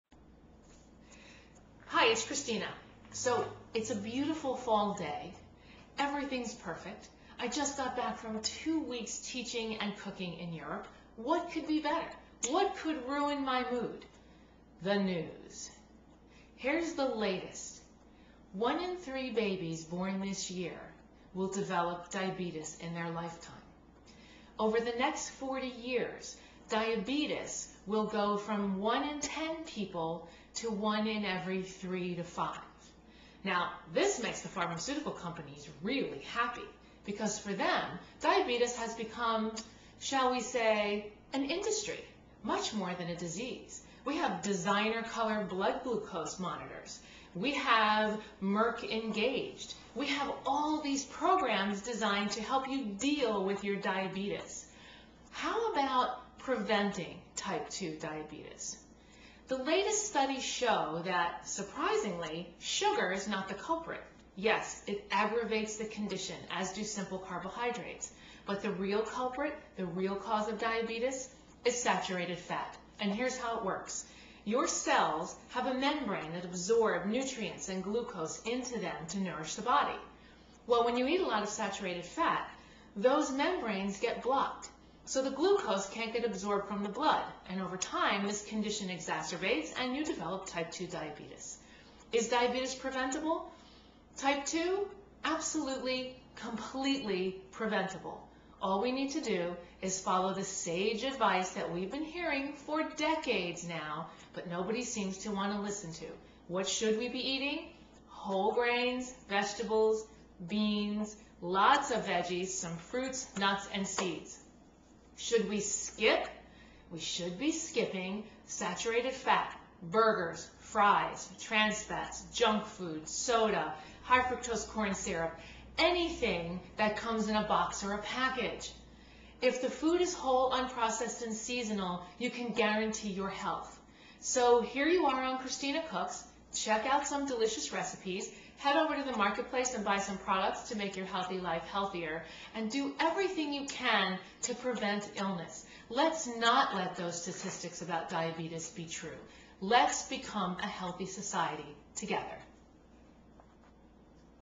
Christina On Diabetes
Christina Pirello, host of the PBS show Christina Cooks, discusses diabetes.